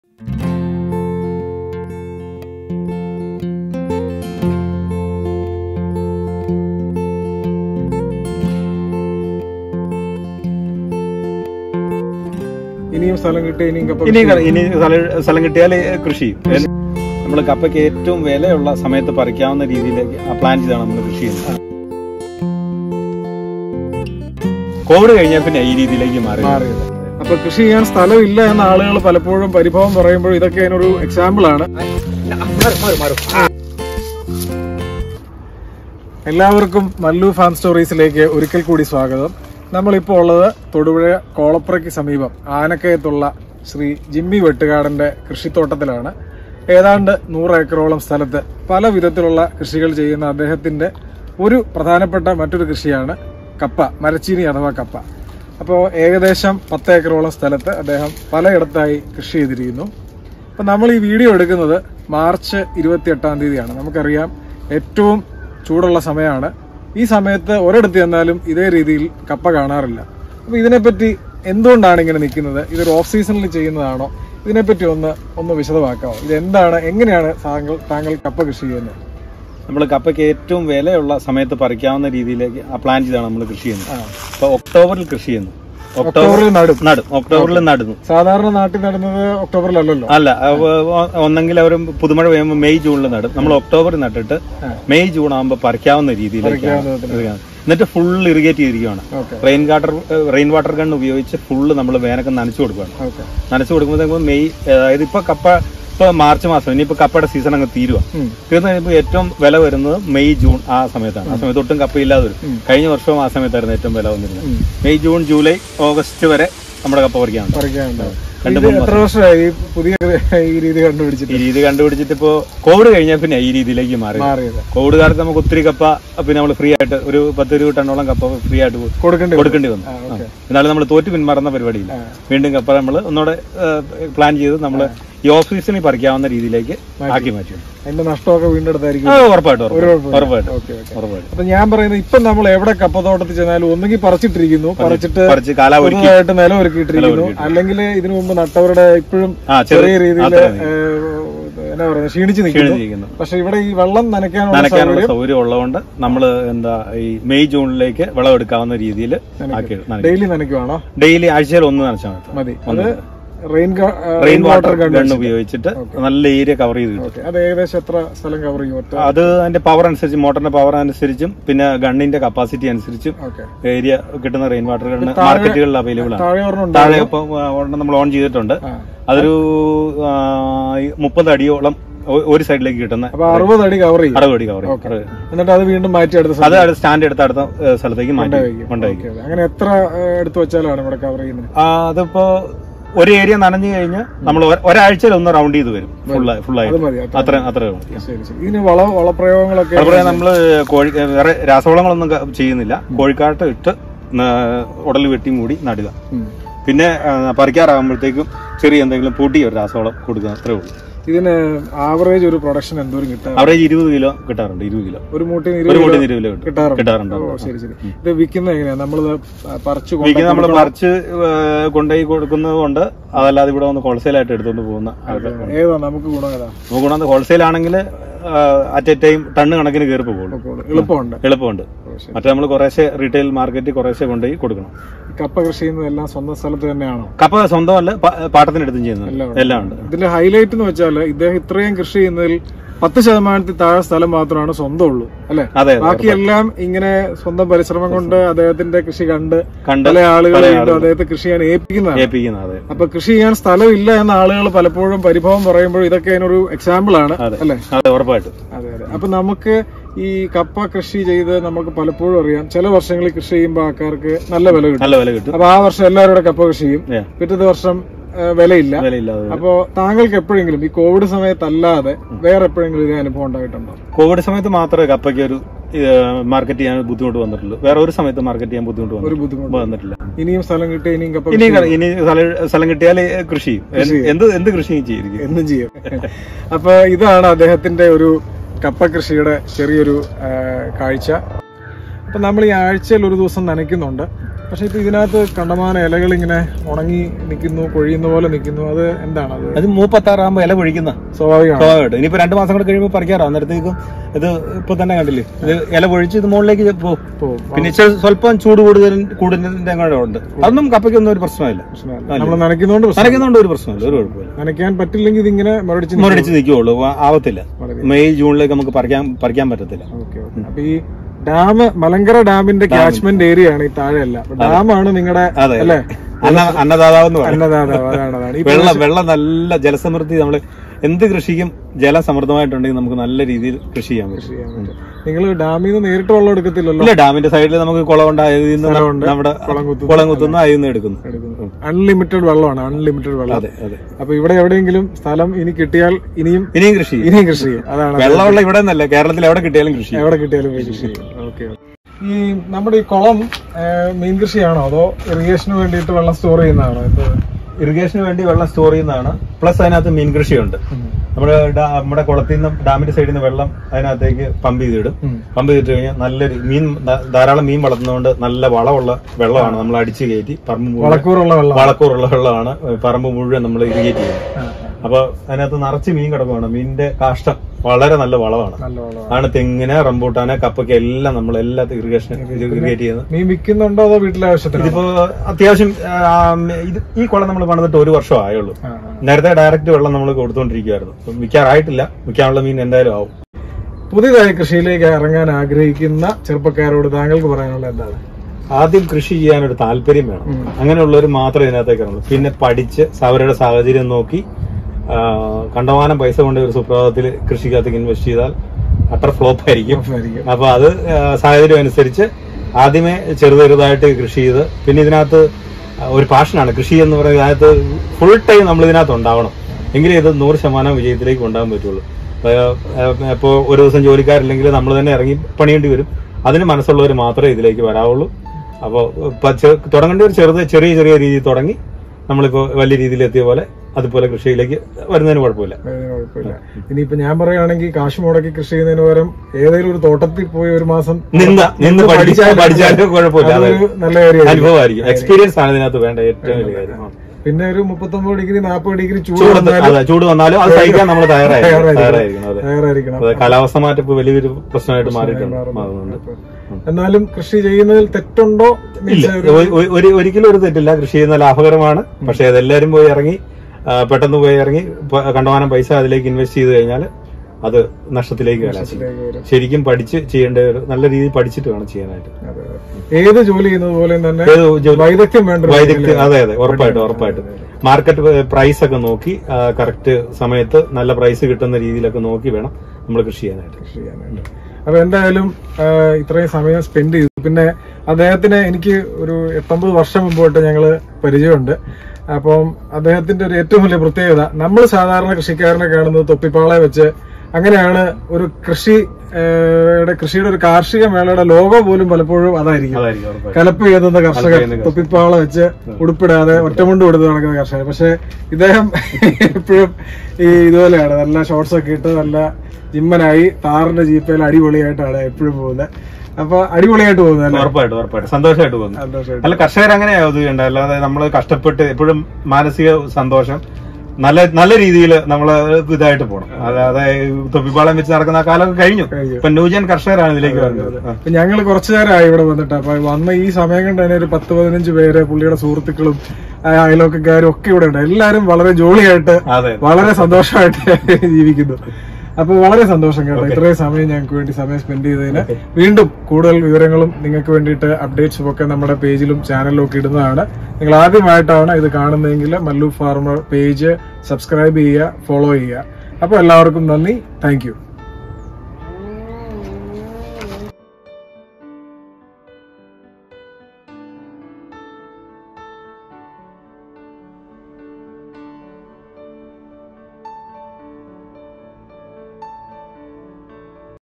100 ഏക്കറിൽ വിജയകരമായ കൃഷി :- Part 2
പാട്ടത്തിന് എടുത്തും സ്വന്തം സ്ഥലത്തും ഉൾപ്പെടെ ഏകദേശം 100 ഏക്കർ സ്ഥലത്തു വിജയകരമായി കൃഷി ചെയ്തു പോരുന്ന ശ്രി ജിമ്മി വെട്ടുകാടന്റെ കൃഷി പരിചയപ്പെടാം

റബ്ബറും റമ്പുട്ടാനും പൈനാപ്പിളും ധാരാളമായി കൃഷി ചെയ്യുന്ന അദ്ദേഹം 10 ഏക്കറോളം മരചീനിയും വിജയകരമായി കൃഷി ചെയ്യുന്നു

എല്ലാവരും കപ്പ നടുന്നതിനു വിപരീതമായി ഓഫ്‌സീസൺ നോക്കിയുള്ള അദ്ദേഹത്തിന്റെ കൃഷി മികച്ച ലാഭം ഉറപ്പു വരുത്തുന്നു

രണ്ടാം ഭാഗം വീഡിയോ കാണാം